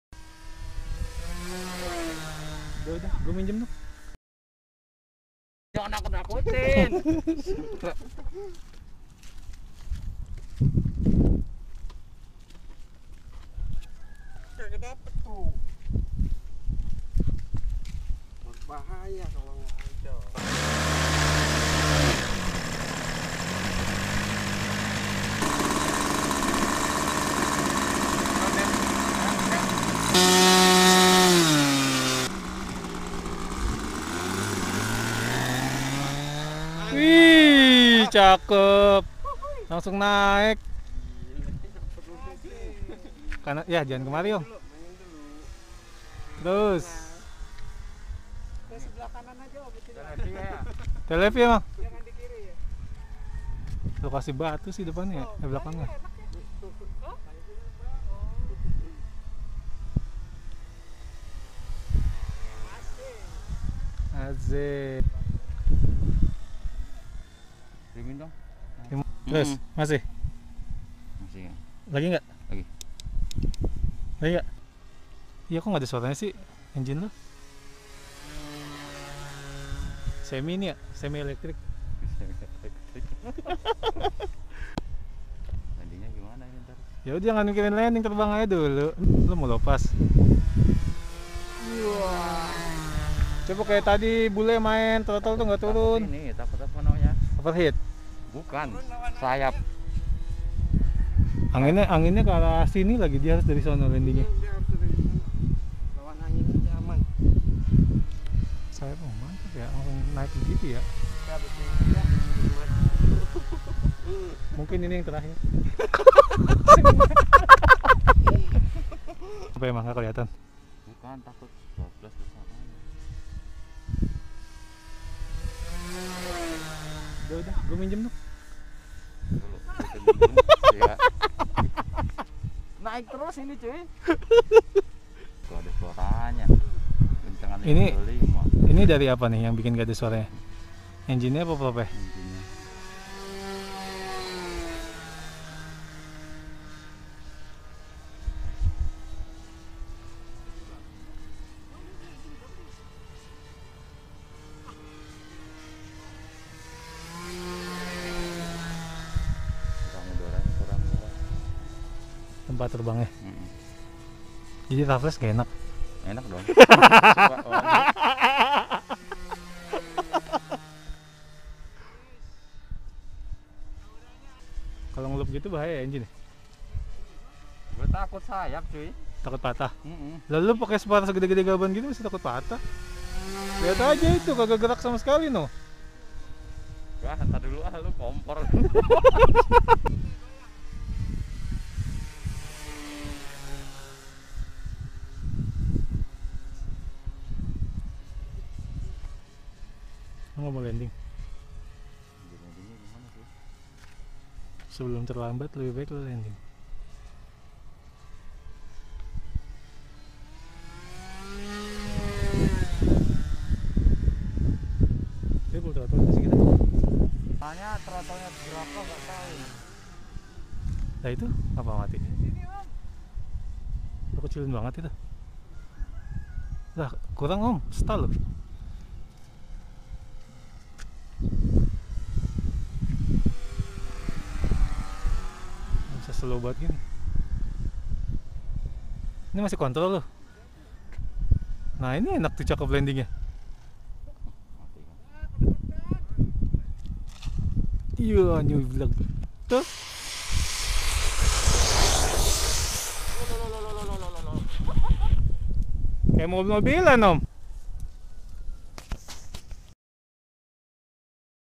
Uji Coba Penerbangan Pesawat RC DIY Bermesin | Maiden flight home Made RC Plane Running Engine OS 46
Pesawat DIY dengan bahan baku tirplek dan polyfoam dengan bentang sayap 2 meter. Plane Made out Of Foamboard, plywood, and balsa